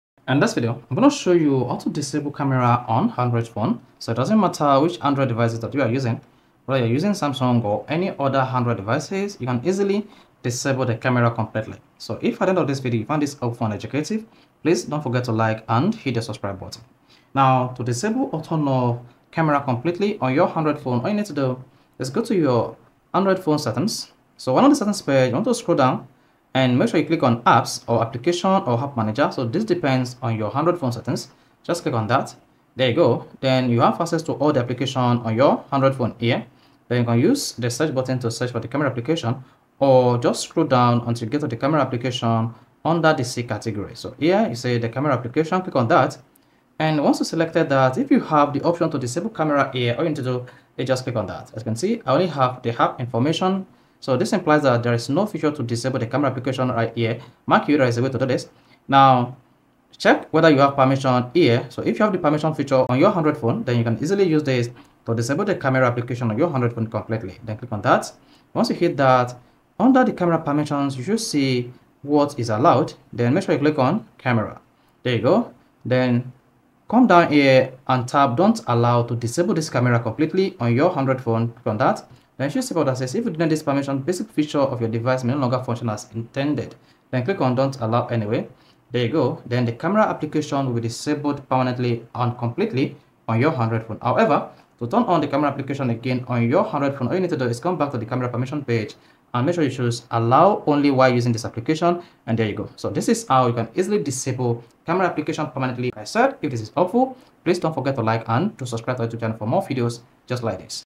How to Disable Camera App on Android Phone
You can disable the camera app on your Android phone to disable app permission on it. You can also turn it on whenever you want to use it. In this video, you will learn how to disable camera app on Android phone.